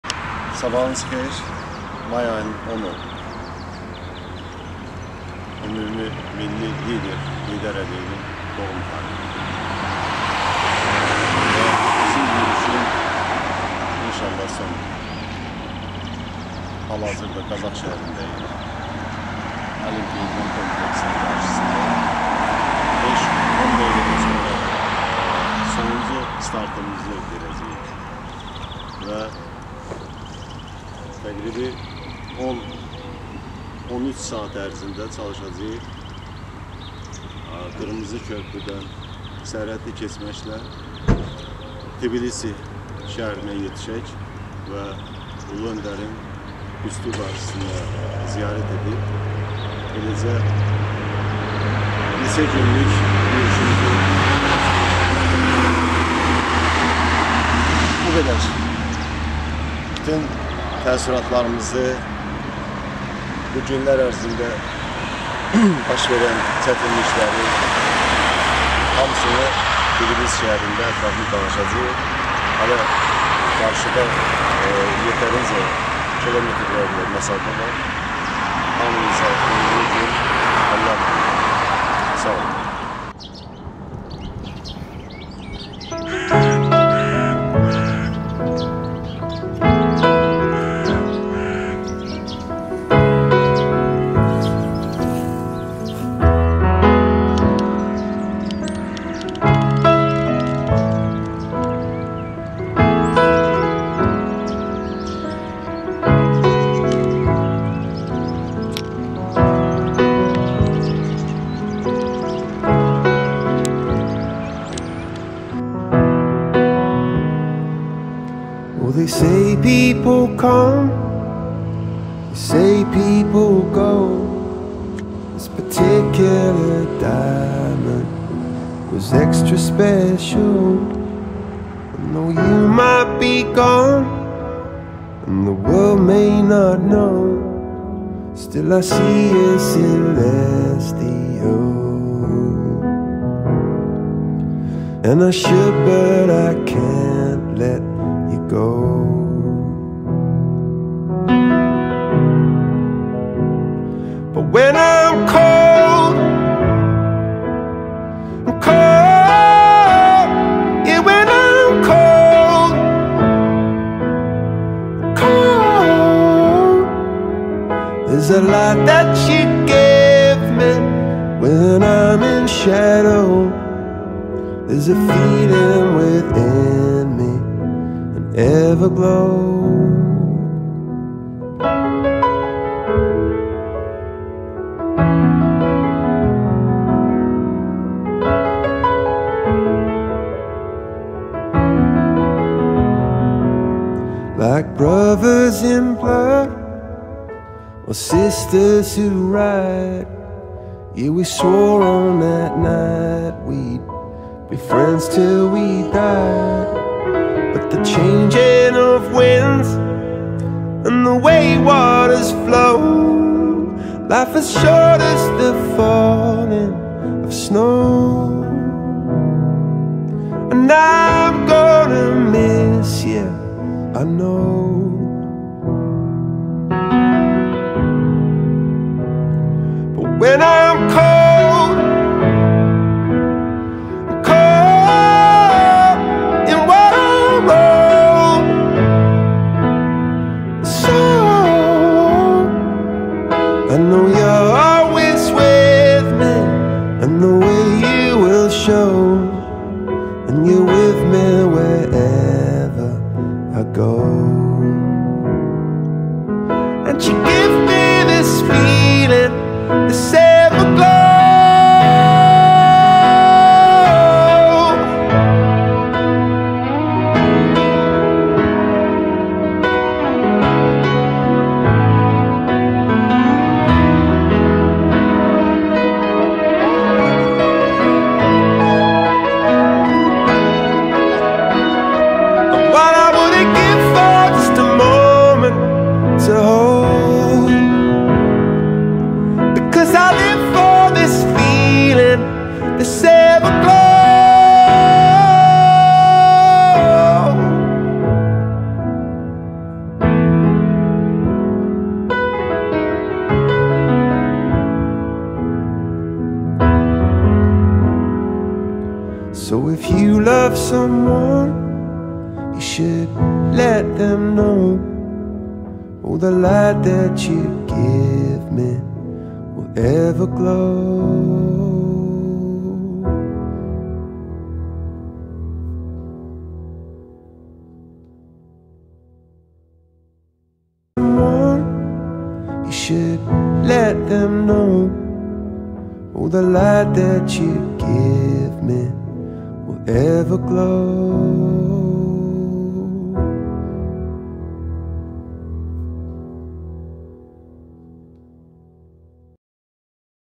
6th Day GAZAKH - RED BRIDGE AZERBAIJAN 10 May 2019
ULTRAMARATHON IN MEMORY OF HEYDAR ALIYEV.
BAKU-TBILISI 544 km
5-10 May 2019.
6th day.Gazakh-Red Bridge Azerbaijan